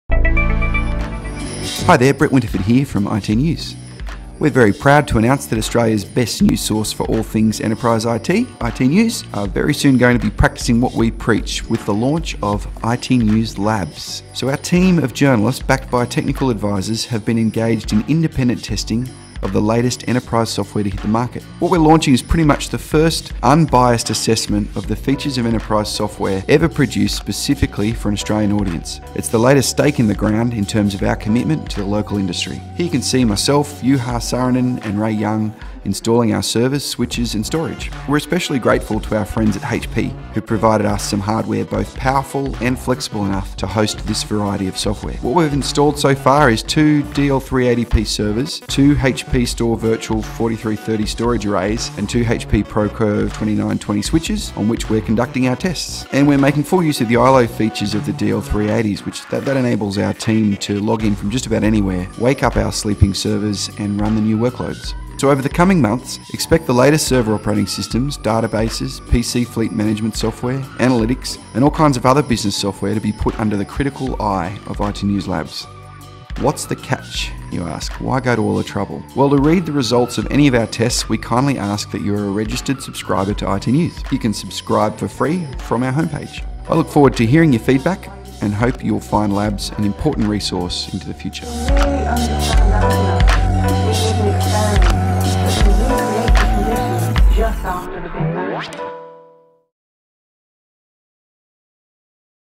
Introducing iTnews Labs
See a timelapse of the iTnews labs being unboxed, set up and switched on!
iTnews will produce independent testing of the latest enterprise software to hit the market after installing a purpose-built test lab in Sydney.
Watch the installation of two DL380p servers, two HP StoreVirtual 4330 storage arrays and two HP ProCurve 2920 switches.